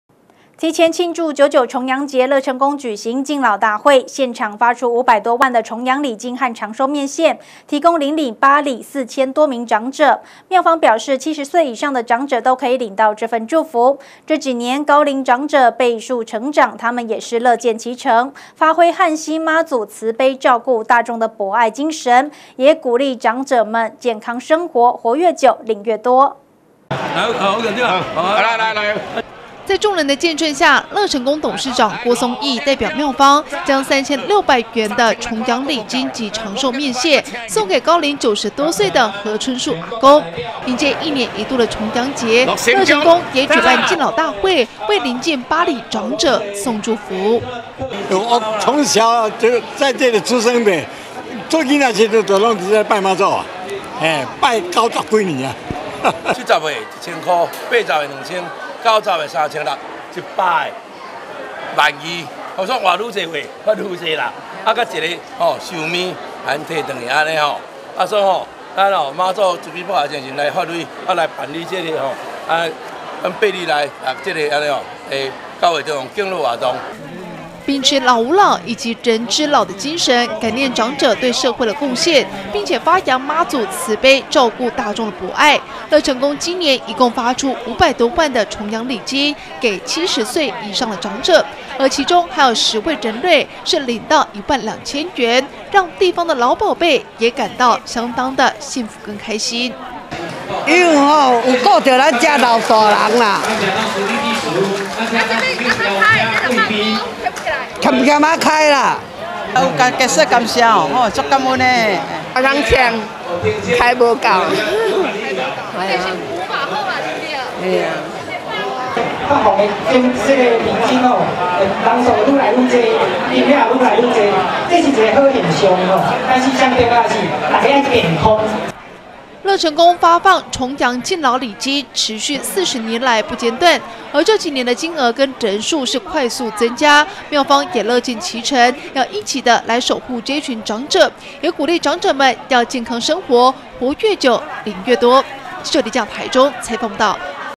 大台中新聞 老大人好幸福 樂成宮發放500萬敬老禮金
提前慶祝九九重陽節，樂成宮舉行敬老大會，現場發出500多萬的重陽禮金及長壽麵線，給鄰近8里4000多名長者，廟方表示，70歲以上的長者都可領到這份祝福，這幾年高齡長者倍數成長，他們樂見其成，發揚旱溪媽祖慈悲照顧大眾的博愛，也鼓勵長者們健康生活，活越久領越多。